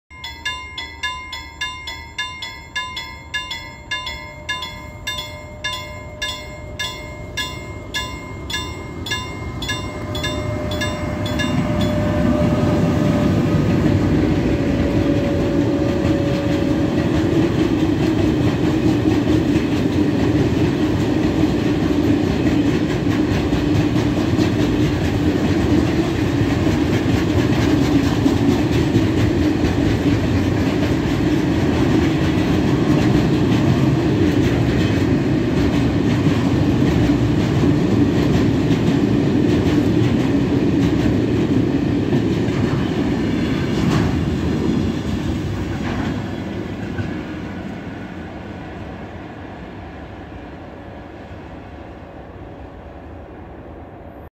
T 58001 Postlarinkadulla
T 58001 Sköldvikistä Kouvola Tavaraan ohittaa Postlarinkadun tasoristeyksen. Sr1+18 Soek+7 Sonk/Zagkks-z+1 Hkba+Vof/Vofa (409m). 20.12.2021.